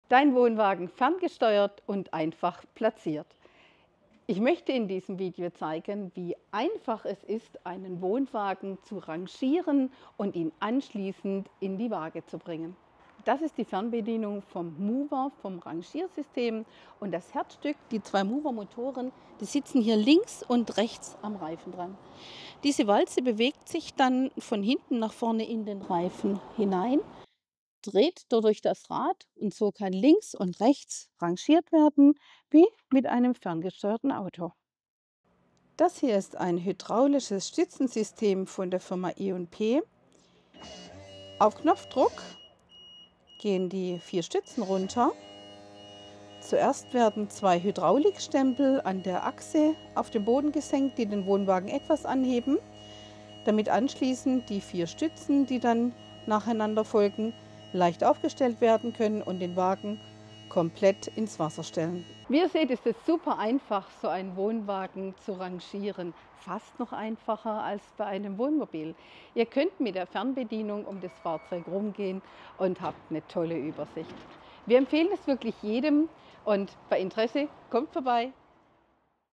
Wohnwagen rangieren leicht gemacht!
Mit einem Mover wird das Rangieren deines Wohnwagens zum Kinderspiel! Egal, wie eng der Stellplatz ist oder wie uneben der Untergrund – du manövrierst deinen Wohnwagen mühelos und präzise genau dorthin, wo du ihn haben möchtest. Kein Schieben oder Kräftemessen mehr, sondern einfach per Fernbedienung steuern!

Mit einem hydraulischen Stützensystem bringst du deinen Wohnwagen nicht nur perfekt ins Wasser, sondern sorgst auch für optimalen Stand auf jedem Untergrund. So wird dein Camping-Erlebnis noch entspannter und komfortabler!

Mach dir das Leben leichter und erlebe, wie unkompliziert Camping sein kann – mit den richtigen Tools von Brecht Caravan!

Jetzt beraten lassen und aufrüsten!